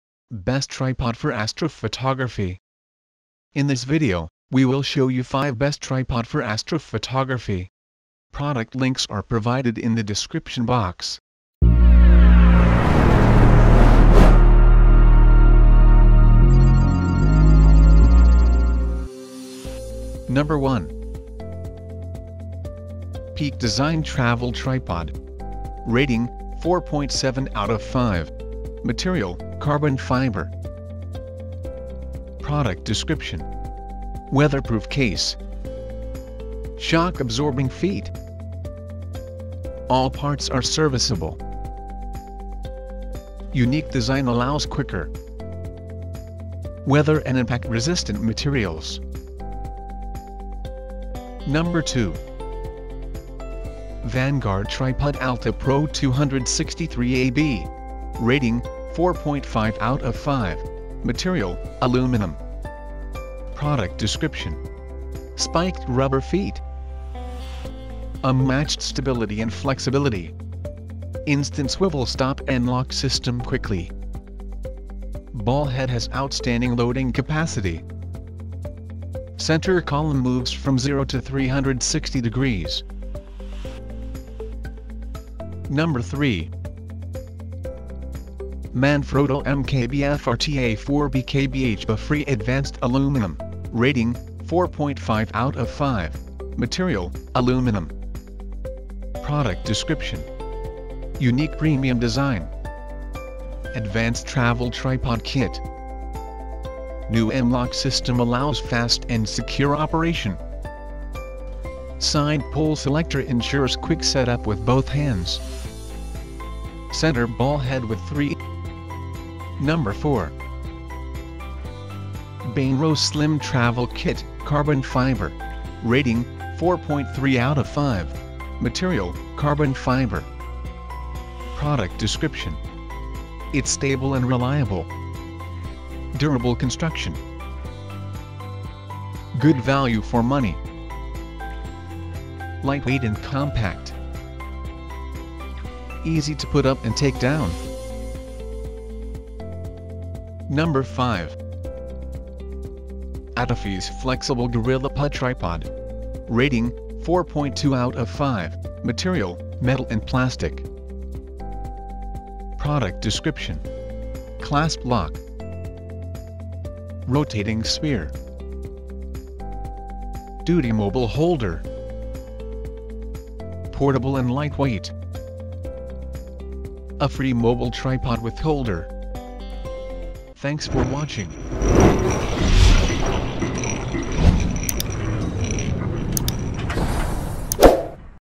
5 Best Tripod For Astrophotography In 2023 - Best Astrophotography Tripod Reviews
When you are looking for the best tripod for astrophotography, kindly make sure to check our roundup. These will help you to get a better shooting experience. Each key info is provided with each product.

All these options might seem a bit hard to grasp. This is why you should check out the reviewers as well. We have included the star rating to help you out in the process.

If you liked the video about the best astrophotography tripod reviews, make sure you press the like button.